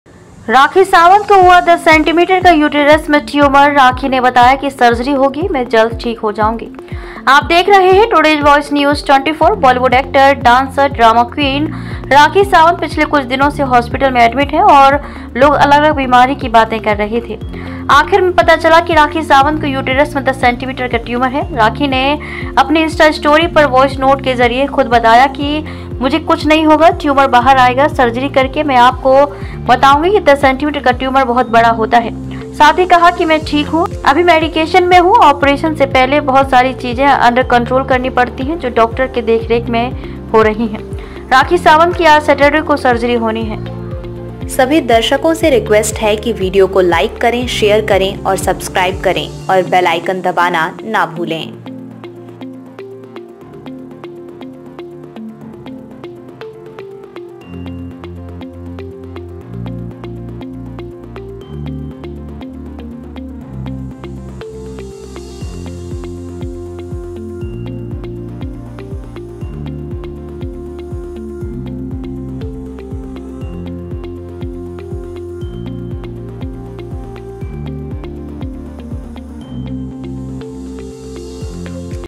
Rakhi Sawant Ko Huwa 10 cm Ka Uterus Mein Tumour, Rakhi Ne Baya Surgery Hogi Main Jald Theek Hojaogi
Rakhi Sawant Ko Huwa 10 cm Ka Uterus Mein Tumour, Rakhi Ne Baya Surgery Hogi Main Jald Theek Hojaongi

BollywoodActor, Dancer, DramaQueenRakhiSawant Pichle Kuch Dino Se Hospital Mein Admit Hain, Aur Log Alag-Alag Beemari Ki Baatein Kar Rahe The.

Aakhir Mein Pata Chala Ke Rakhi Sawant Ko Uterus Mein 10 cm Ka Tumour Hai.

Rakhi Ne Apne Insta Story Par Voice Note Ke Zariye Khud Bataya Ke Mujhe Kuch Nahi Hoga, Tumour Bahar Aayega Surgery Karke, Main Aapko Bataongi. 10 cm Ka Tumour Bahut Bada Hota Hai.

Sath Hi Kaha Ke Main Theek Hoon, Abhi Medication Mein Hoon. Operation Se Pehle Bahut Sari Cheezen Under Control Karni Padti Hain, Jo Doctor Ke Dekh Rekh Mein Horahi Hai.

Rakhi Sawant Ki Aaj Saturday Ko Surgery Honi Thi.

Prayers ForRakhi